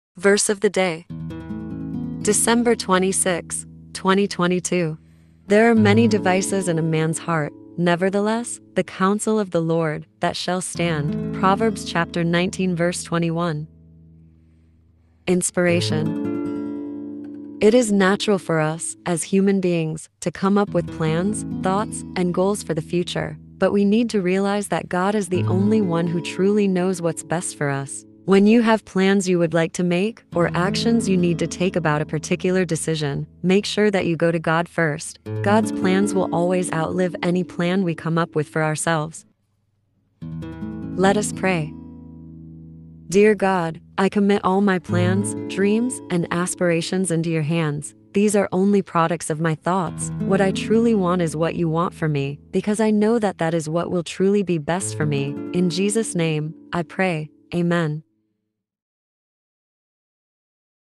BIBLE VERSE OF THE DAY - December 26, 2022 | morning prayer | Your Daily Dose of God's Word.
There are many devices in a man's heart; nevertheless the counsel of the LORD, that shall stand.
-Proverbs 19:21

INSPIRATION:
It is natural for us, as human beings, to come up with plans, thoughts, and goals for the future. But we need to realize that God is the only one who truly knows what's best for us. When you have plans you would like to make, or actions you need to take about a particular decision, make sure that you go to God first. God's plans will always outlive any plan we come up with for ourselves.

PRAYER:
Dear God, I commit all my plans, dreams, and aspirations into your hands. These are only products of my thoughts; what I truly want is what you want for me because I know that that is what will truly be best for me. In Jesus' name, I pray. Amen.

us